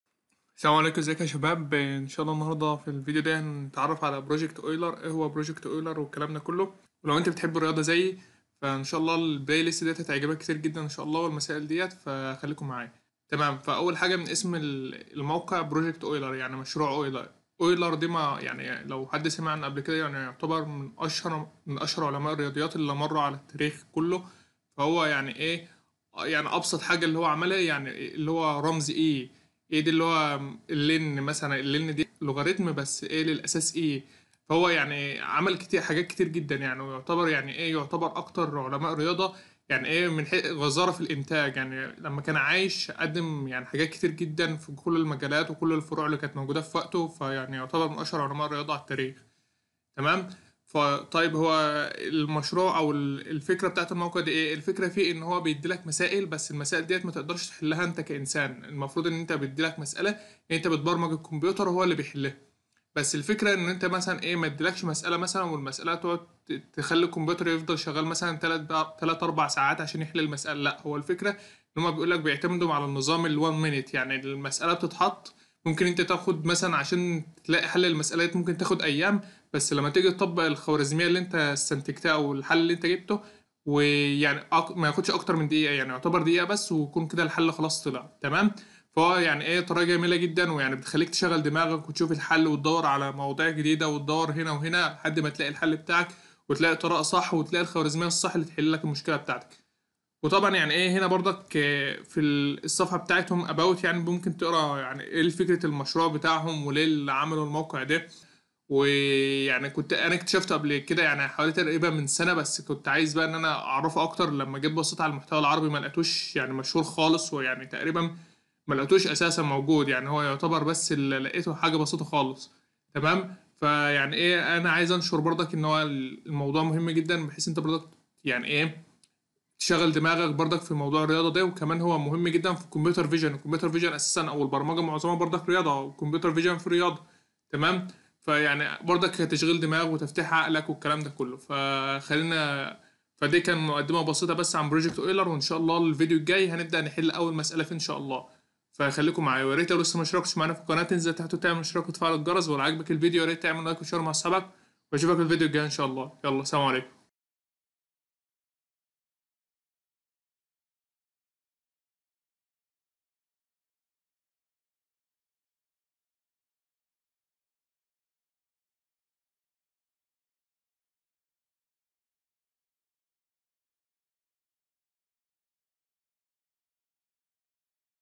ما هو مشروع أويلر ؟ || ? What's Project Euler
السلام عليكم يا شباب الفيديو ده عبارة عن مقدمة لمشروع أويلر
هنتكلم عن فكرة الموقع وليه معروف بالاسم ده وايه الهدف منه

يا رب الفيديو يعجبكم وماتنساش تعمل اشتراك  🔥🔥
 عشان يوصلك كل جديد وشير الفيديو عشان المنفعة تعم